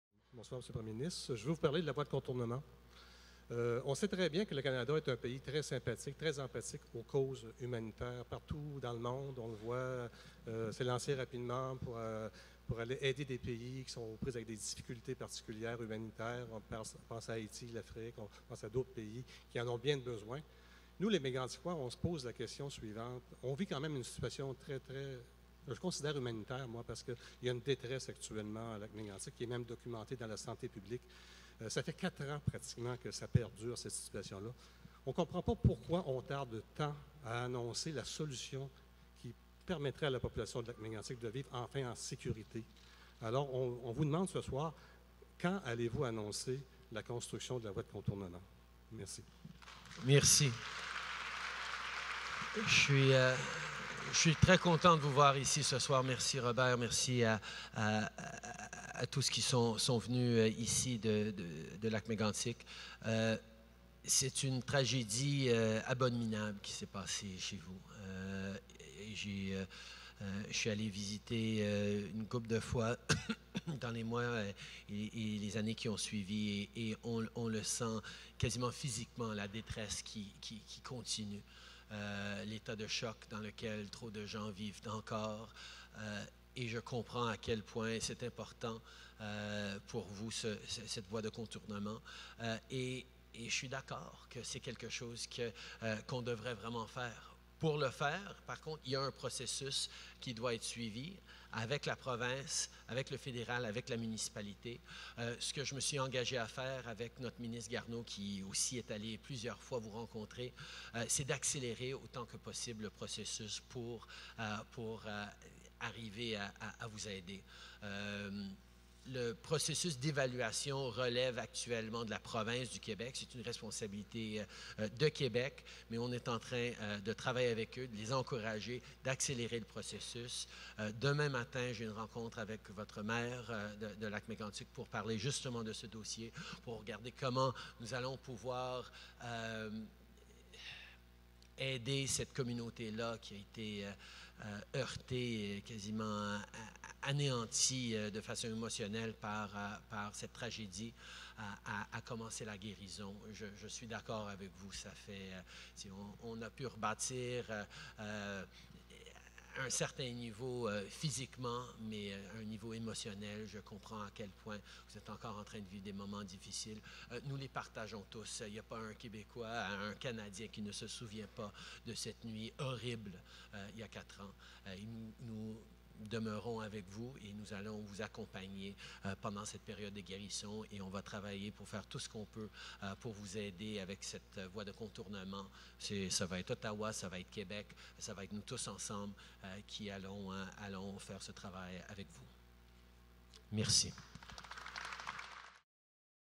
PM Trudeau discusses rail safety in Lac-Mégantic during a town hall in Sherbrooke, Quebec
Prime Minister Justin Trudeau discusses rail safety in Lac-Mégantic during a town hall in Sherbrooke, Quebec